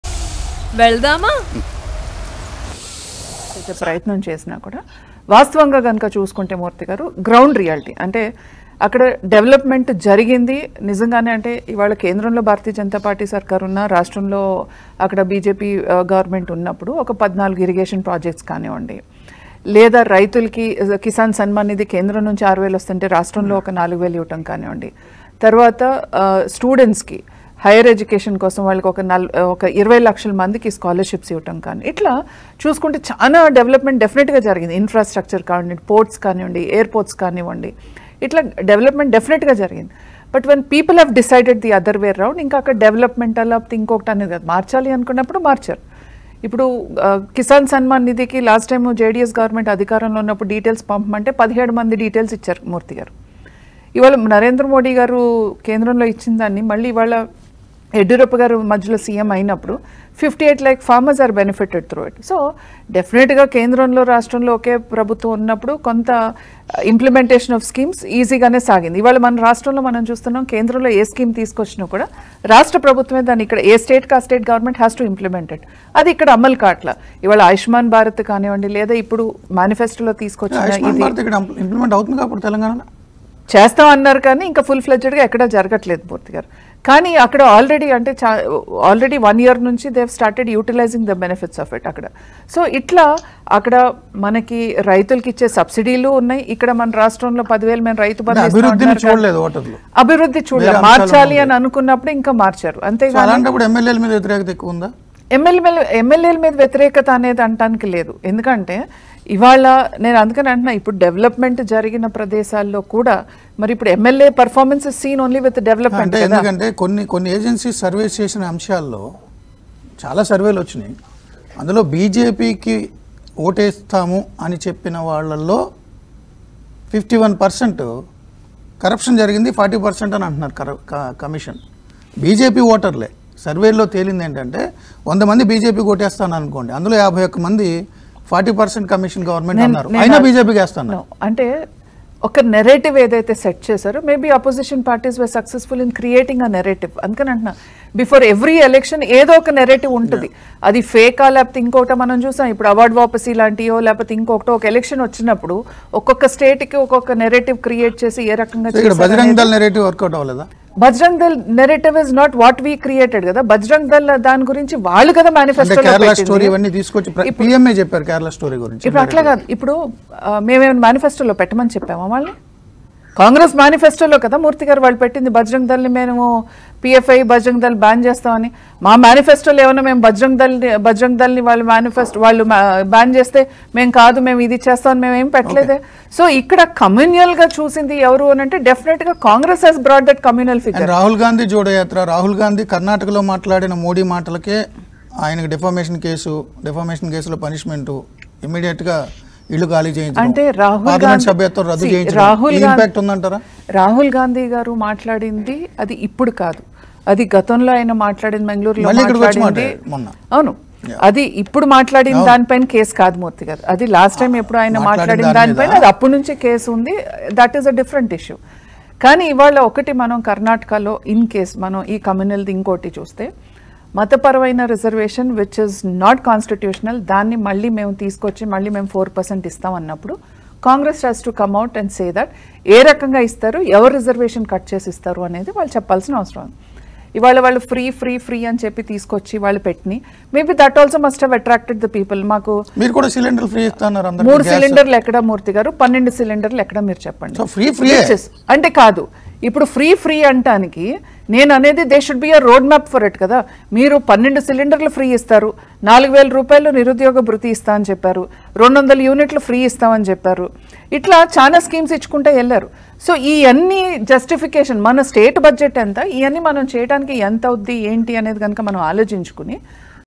బజరంగ్‌ దళ్‌పై బీజేపీ నేత మాధవి సంచలన కామెంట్స్ BJP Leader Madhavi About Bajrang Dal | Tv5 News
LIVE: క‌ర్నాట‌కలో కాంగ్రెస్ విజయం...? | Karnataka Election Results 2023 | Karnataka Results | TV5